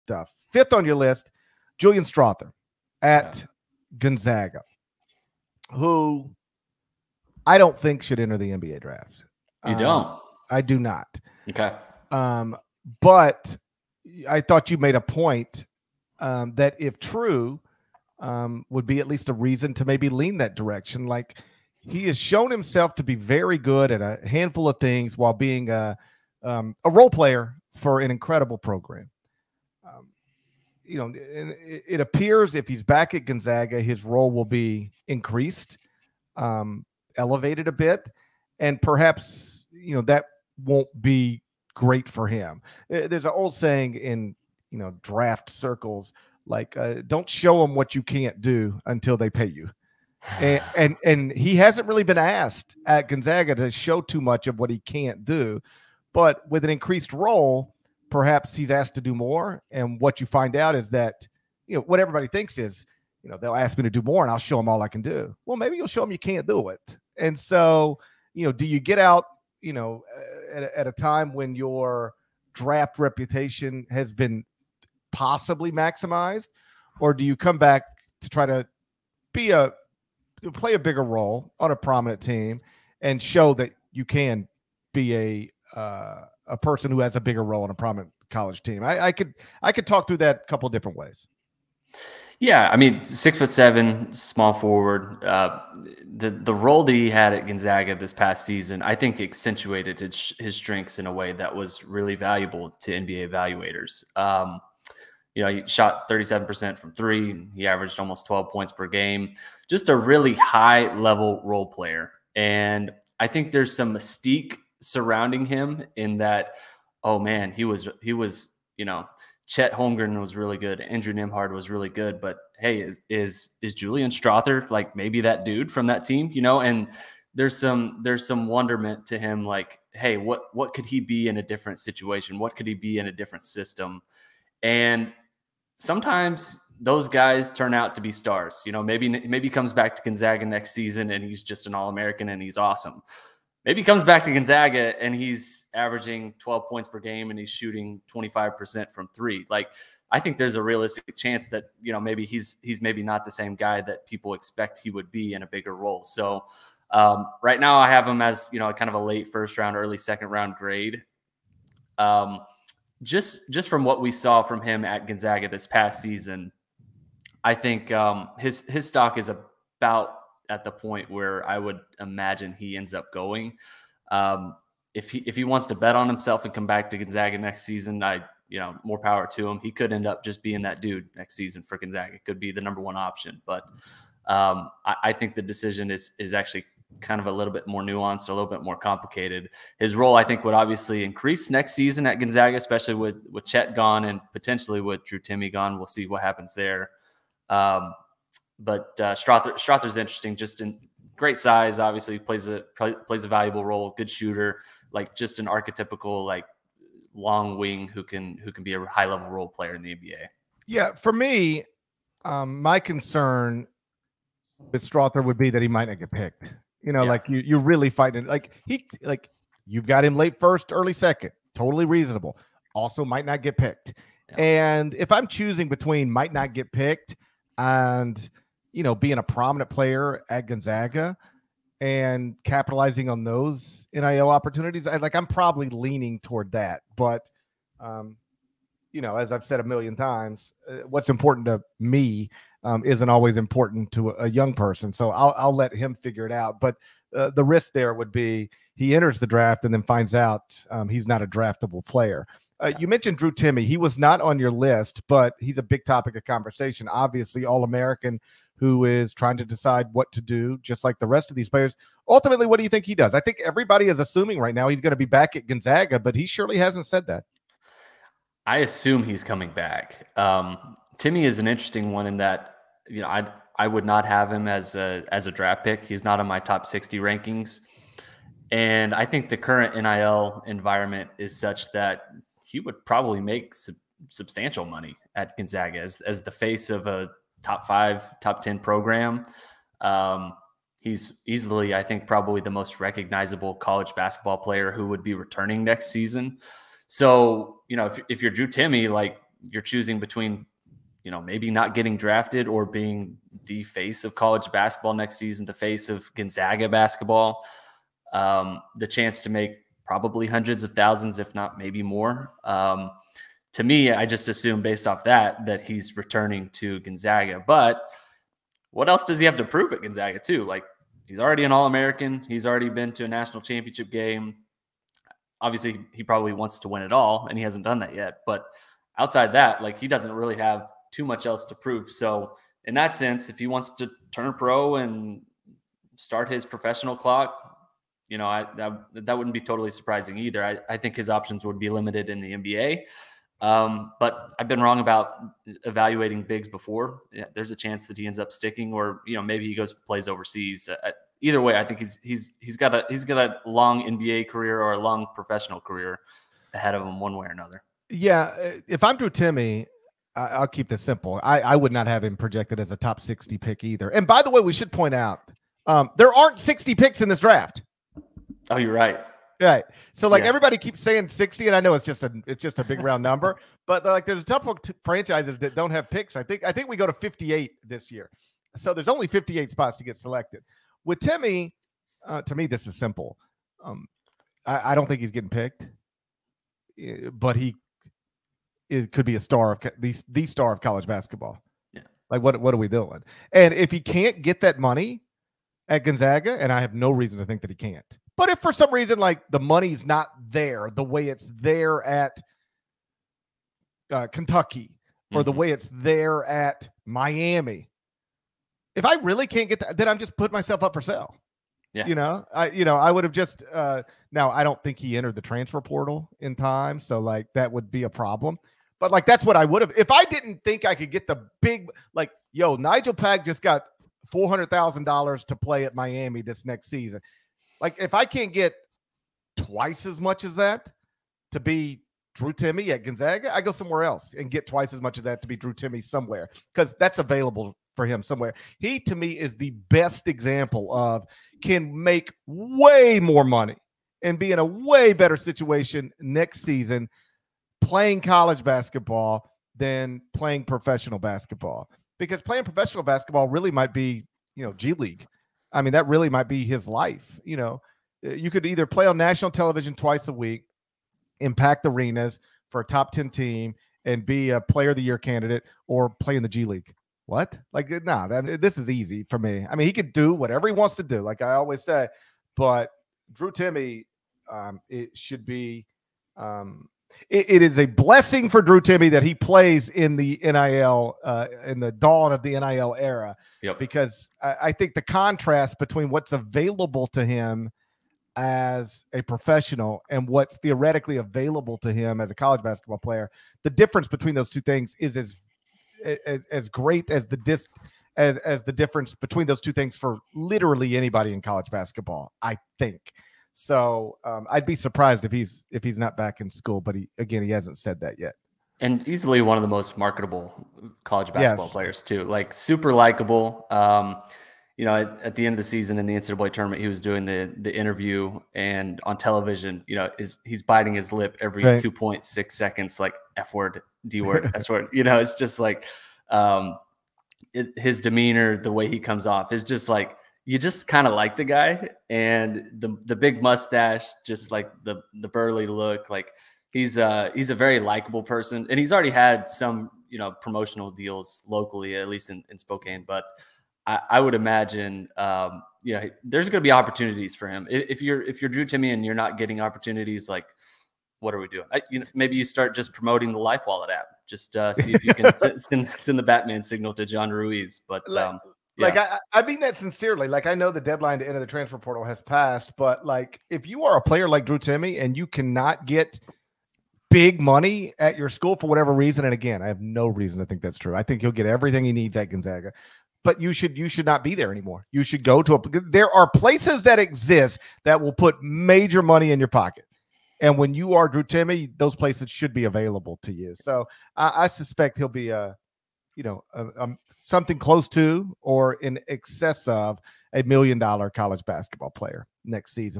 DREW TIMME AND JULIAN STRAWTHER CAN MAKE MORE MONEY IN COLLEGE THAN IN THE NBA | College Basketball
Are Drew Timme's and Julian Strawther's pro prospects strong enough to go to the NBA or should they stay in college? Gary Parrish and Kyle Boone discuss